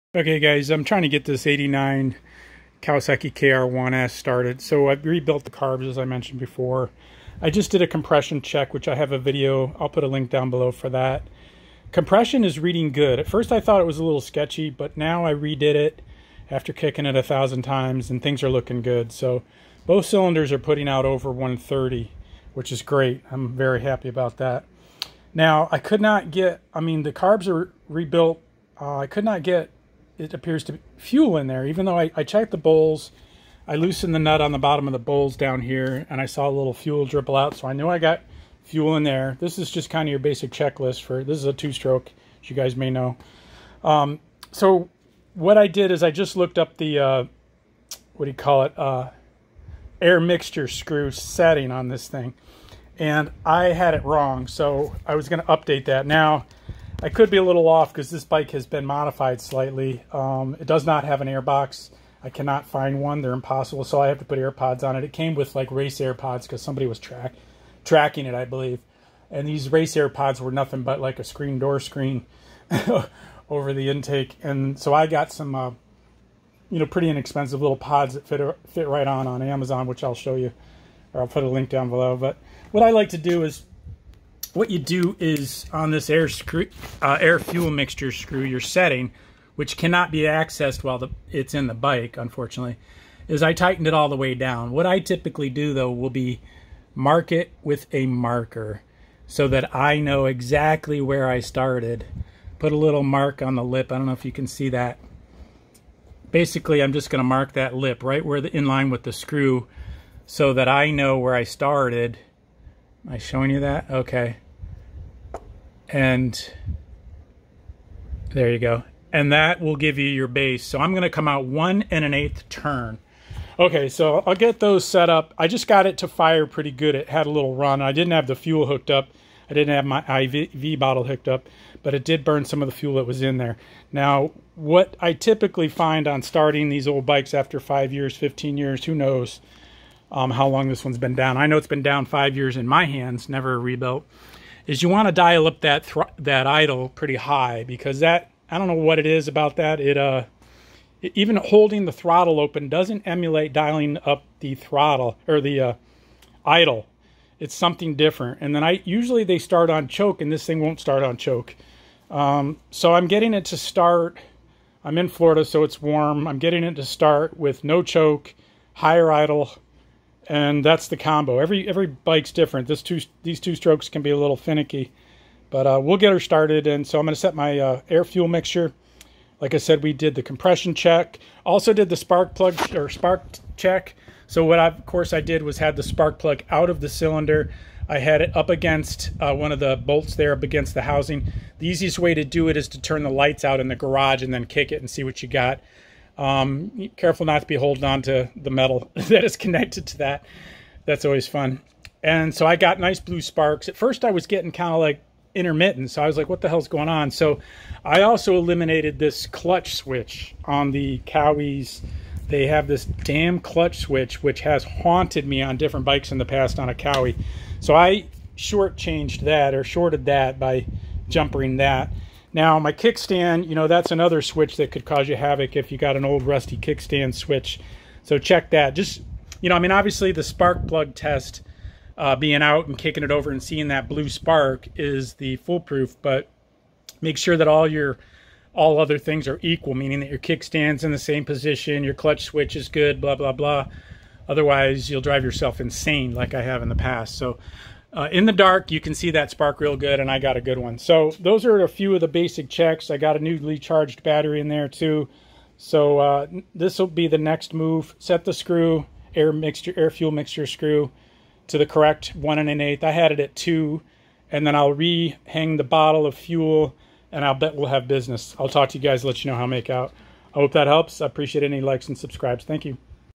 1989 Kawasaki KR-1S 250cc 2-Stroke startup tips.
Old 2 stroke startup check list, tuning tips and troubles. 1989 Kawasaki KR-1S 250cc.  Compression check